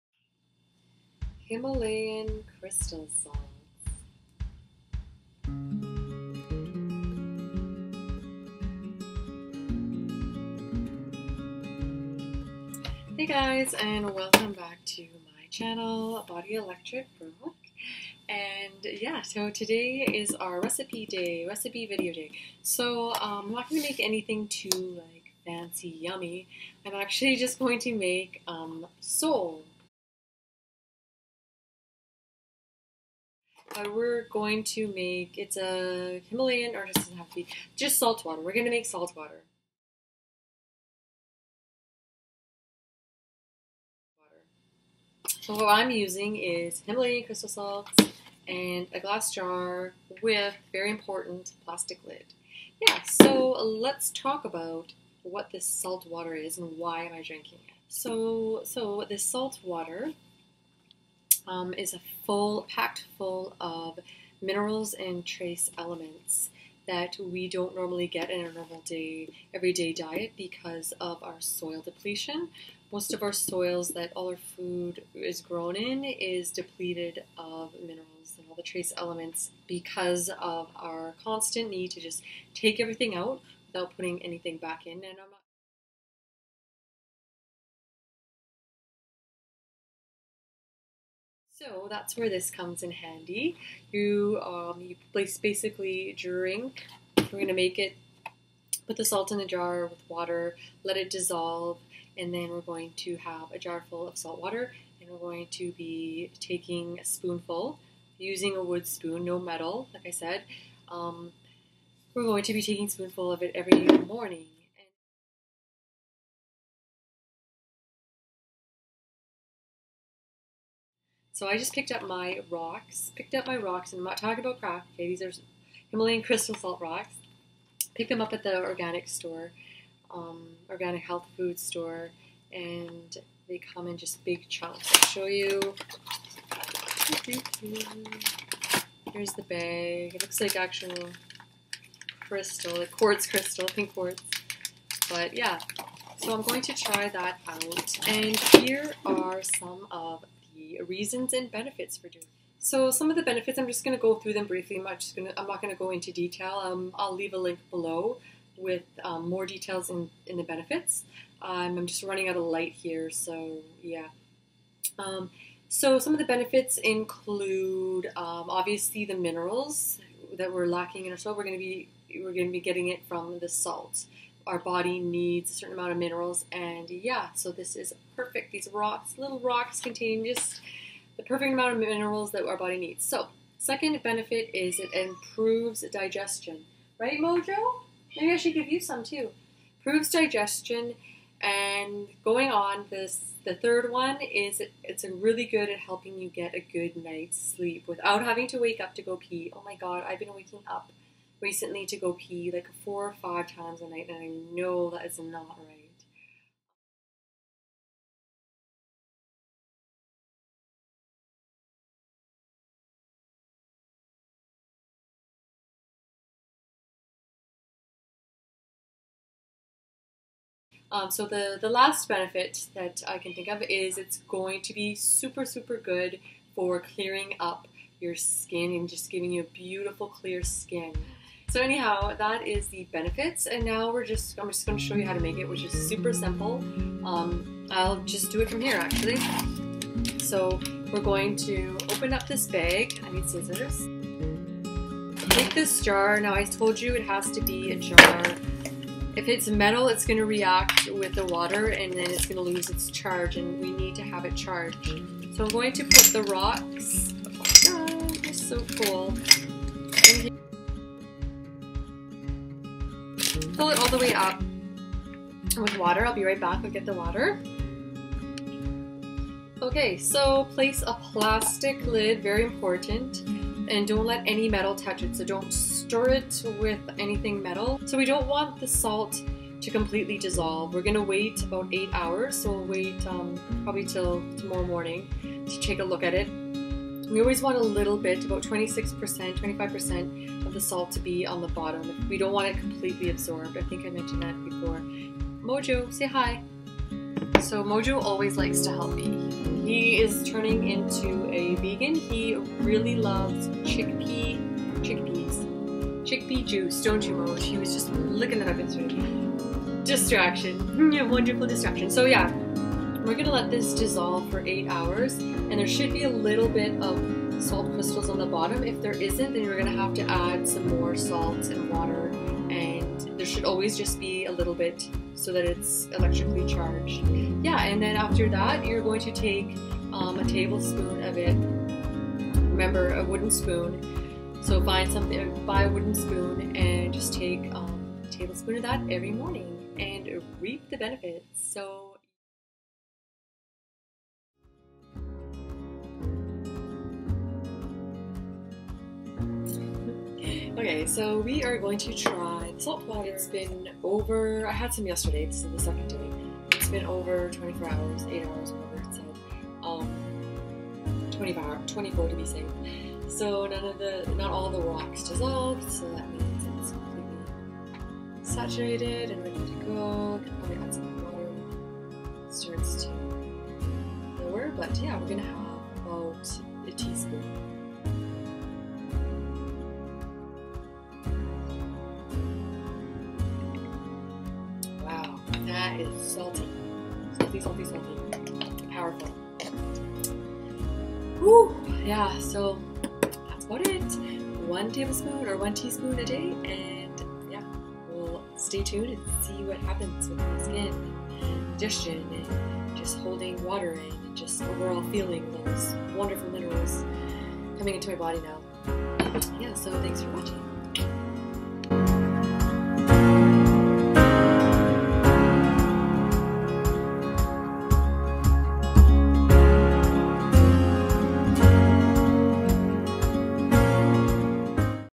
DRINKING SOLE || WHAT IS IT? AND WHY AM I DRINKING IT?
What am I drinking now? Always something right? If there's a real holistic health benefit I will definitely give it a whirl. In today's video I'm making and drinking SOLE.

What is SOLE? Sole is a natural salt water solution full of 84 trace minerals essential for life. Dissolving salt into water creates a new electrical charge, an electrolyte, which has many benefits for the body like helping the kidneys maintain fluid balances.

Super easy to make, all you need is natural salt (either Pink Himalayan Crystal Salt or Sea Salt), spring or filtered water, and a glass jar with a plastic lid.

I'm drinking it every morning now. I'll give it a couple weeks to see if I notice any changes in my body.